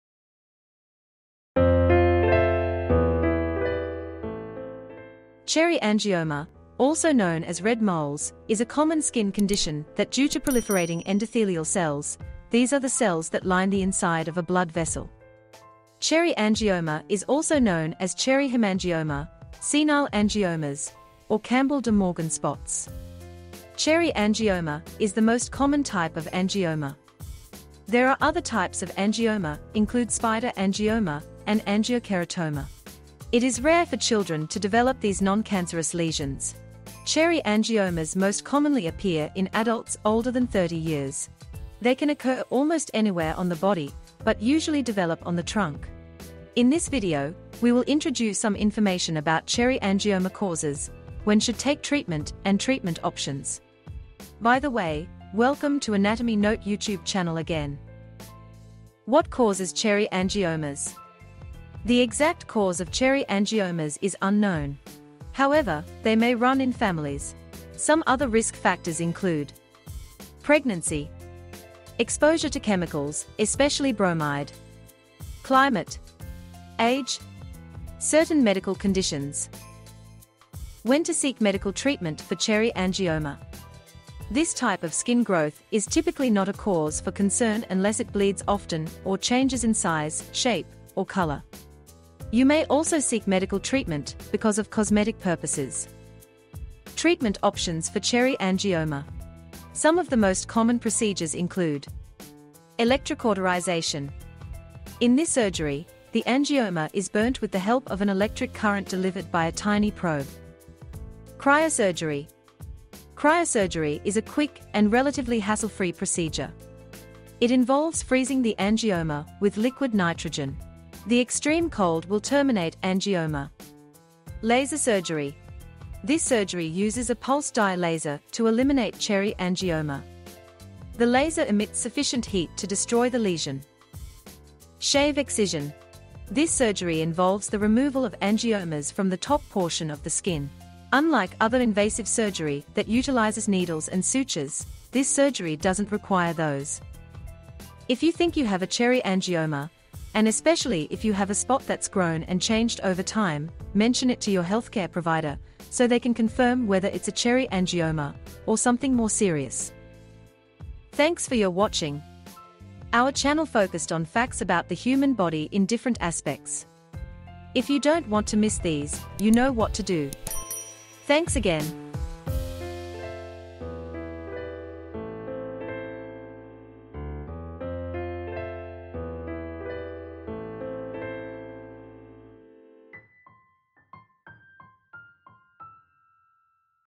Cherry angioma causes, Shapes, and treatment options | Red Mole, Cherry Hemangioma | Mole Removal
Cherry angioma, also known as red moles, is common skin conditions that due to proliferating endothelial cells, these are the cells that line the inside of a blood vessel. Cherry angioma is also known as cherry hemangioma, senile angiomas, or Campbell de Morgan spots. Cherry angioma is the most common type of angioma. there are other types of angioma include spider angioma and angiokeratoma.

It is rare for children to develop these noncancerous lesions. Cherry angiomas most commonly appear in adults older than 30 years. They can occur almost anywhere on the body but usually develop on the trunk.

In this video, we will introduce some information about cherry angioma causes, when should take treatment, and treatment options.

By the way, welcome to Anatomy Note Youtube Channel again.

Chapter:

00:00 Cherry angioma introduction
00:59 Causes and risk factors of cherry angioma
01:21 When to seek medical treatment for cherry angioma?
01:37 Treatment options for cherry angioma
02:41 Thanks, Please like, comment, and subscribe Anatomy note youtube channel

What causes cherry angiomas?

The exact cause of cherry angiomas is unknown. However, they may run in families. Some other risk factors include:

1. Pregnancy
2. Exposure to chemicals, especially bromide
3. Climate
4. Age
5. Certain medical conditions

When to seek medical treatment for cherry angioma?

This type of skin growth is typically not a cause for concern unless it bleeds often or changes in size, shape, or color. You may also seek medical treatment because of cosmetic purpose.

Treatment options for cherry angioma

Some of the most common procedures include:

Electrocauterization:

If you think you have a cherry angioma, and especially if you have a spot that's grown and changed over time, mention it to your healthcare provider so they can confirm whether it's a cherry angioma or something more serious.

☠️ About us
Anatomy is an amazing science. It can help you understand our world more detailed and specific.

Don’t miss

Video credit: Storyblocks
Picture credit: Google